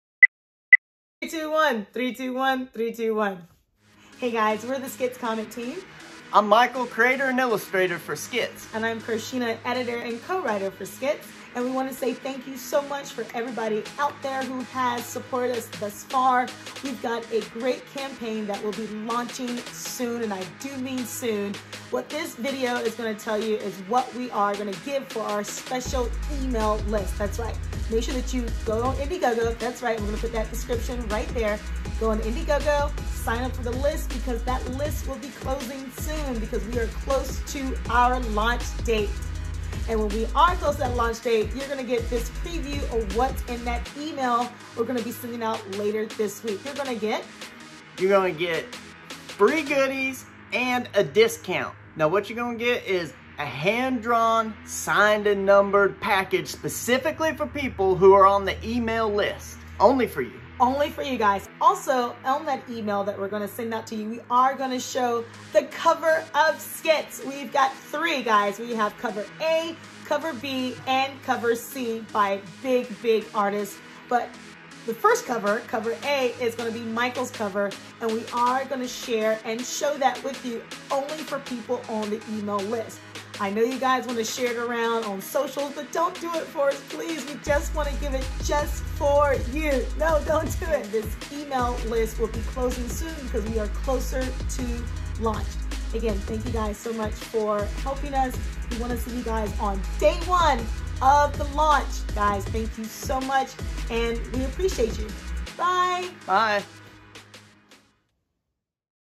SKITS: THE SON, BOOK 1 LAUNCHING SOON ON INDIEGOGO UPDATE VIDEO
SKITS: THE SON, BOOK 1
3 BOOKS, 3 VARIANT COVERS, 3 VARIANT STORIES, 1 COMPLETED JOURNEY
Creator, Illustrator, Writer- Michael Siegenthaler
Editor, Co-Writer- Carshénah Jefferson Siegenthaler